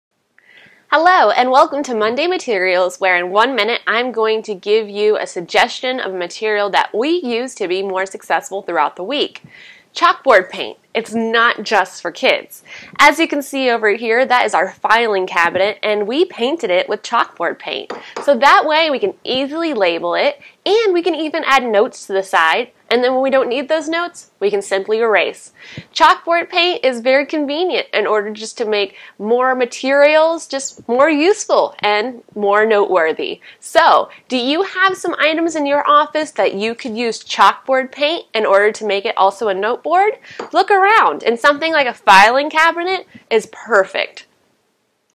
MondayMaterial How To Make Your Office Furniture More Useful & Productive For You
MondayMaterial to help you be more successful throughout the week is about how to make your office furniture more useful and more productive for you. We have so many more great training tips

Chalkboard paint is not just for kids! When we moved into our new office, we painted our filing cabinet with chalkboard paint. This made it easy to label the different sections as well as write notes on the cabinet that can be easily erased. It also looks pretty nice just being black versus the metal.

What do you have in your office that could be more productive for you. I don't suggest your desk only because you need put items on there too often and you have the potential to rub off an important note. That's why a filing cabinet, something fairly stand alone and not touched often is a great furniture piece to do this with. I have seen some people dedicate a whole wall to this stuff in order to use it as a creative space, as well as a brainstorm dump. So, what are you going to cover in Chalkboard paint?